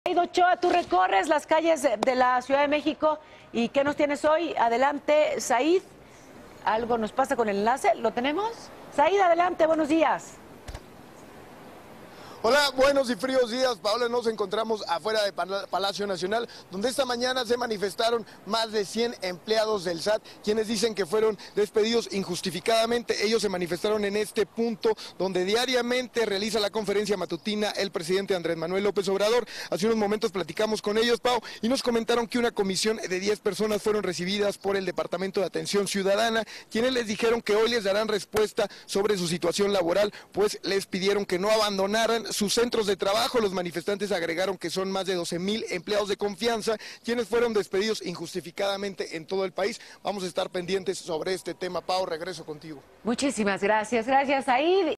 Trabajadores se manifiestan tras ser despedidos del SAT - Al Aire
Tras ser despedidos del SAT, trabajadores se manifiestan las inmediaciones de Palacio Nacional, en la Ciudad de México